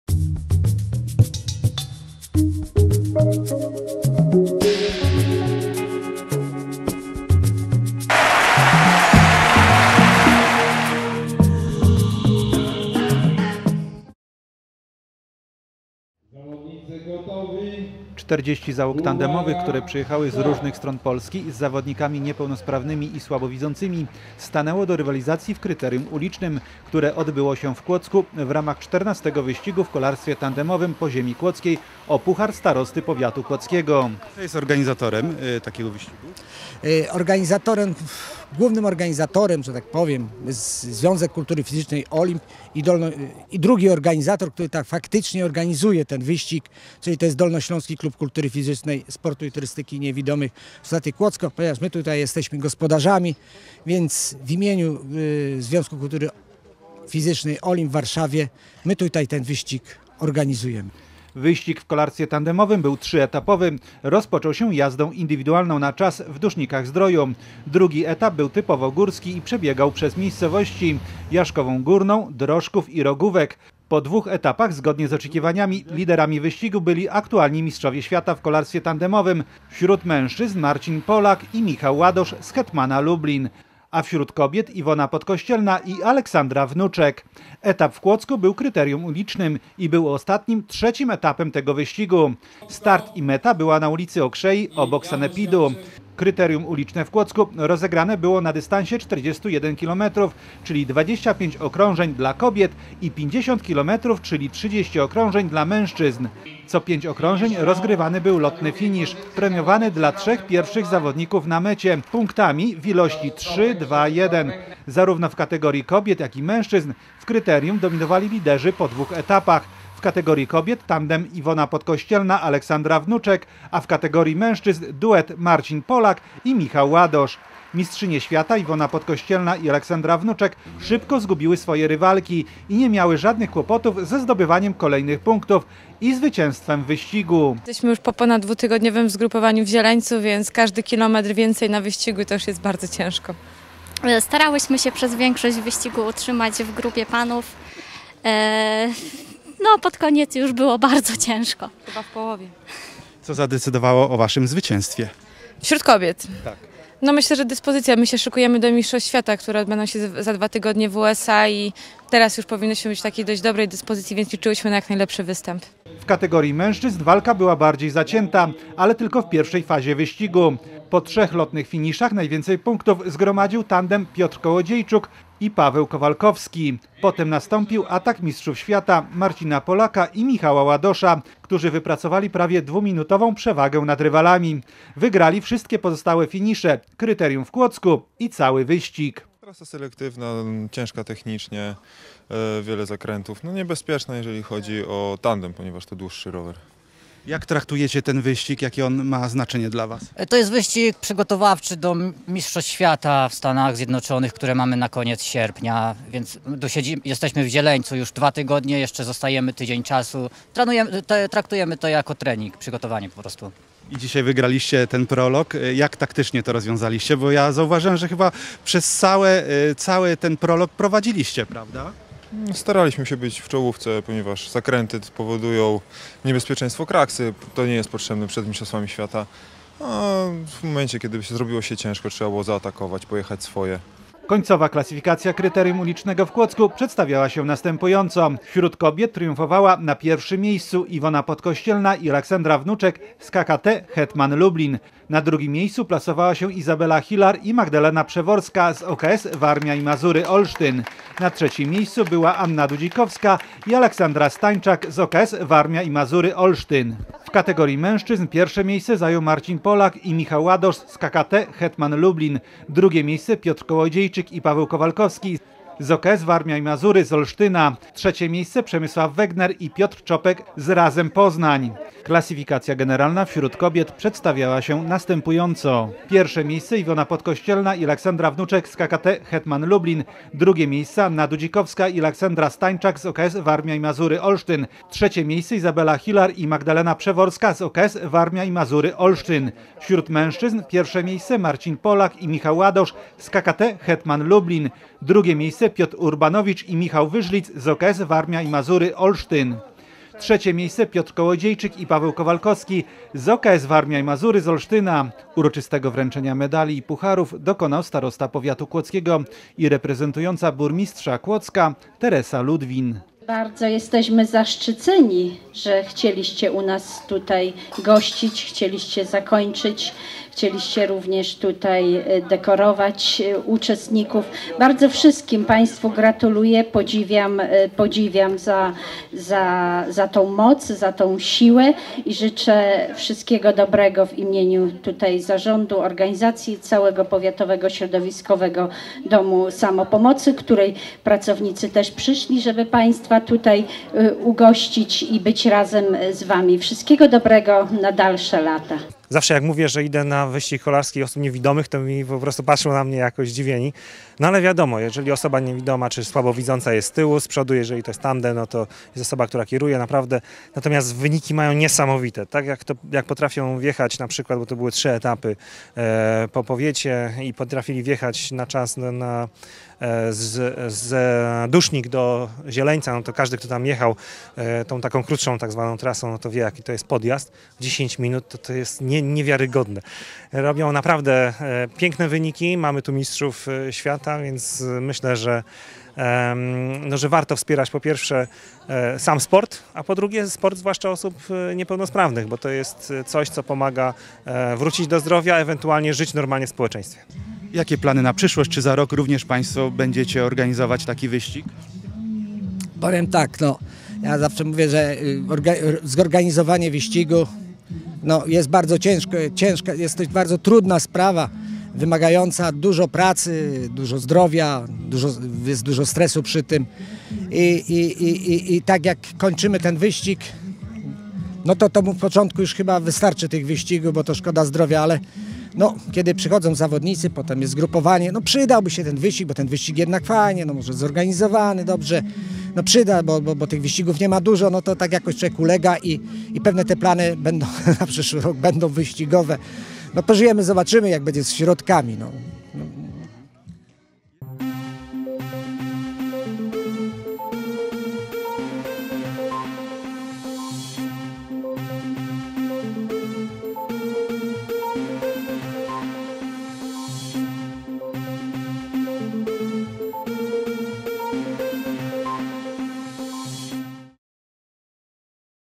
XIV wyścig w kolarstwie tandemowym
W Kłodzku odbyło się kryterium uliczne XIV wyścigu w kolarstwie tandemowym Po Ziemi Kłodzkiej O Puchar Starosty Kłodzkiego.